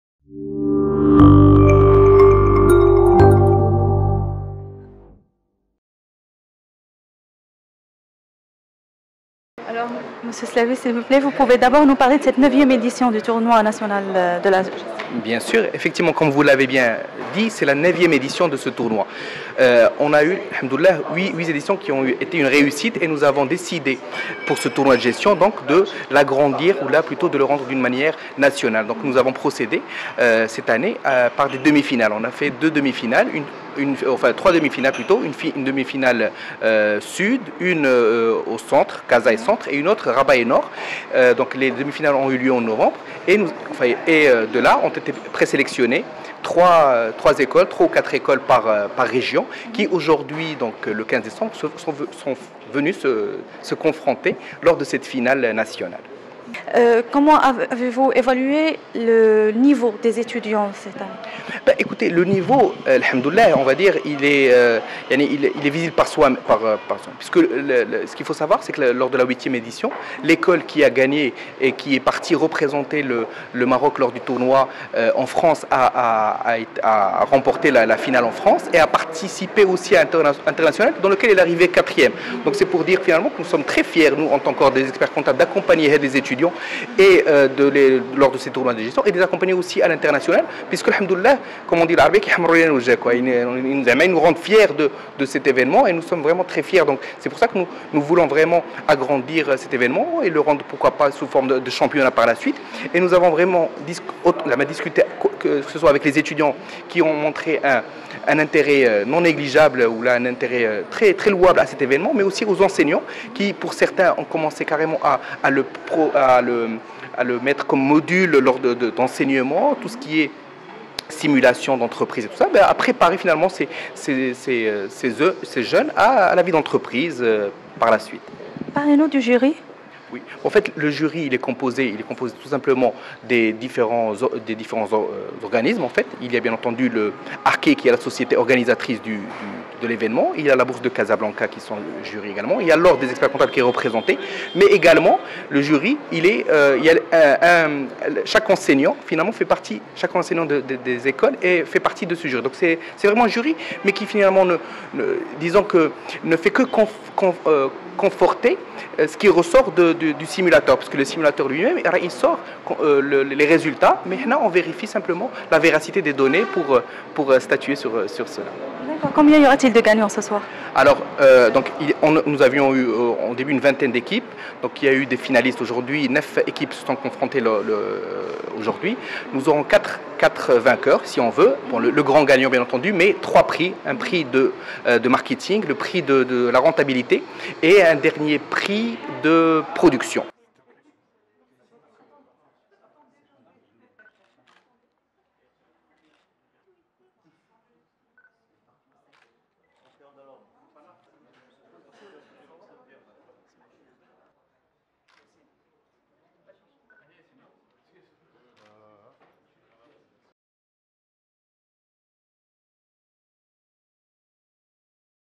Quatre lauréats primés au 9e Tournoi national de la gestion
Le conseil de l’Ordre des experts comptables, en partenariat avec la Bourse de Casablanca, a organisé vendredi, la cérémonie de remise des Prix aux gagnants de la 9e édition du Tournoi national de la gestion.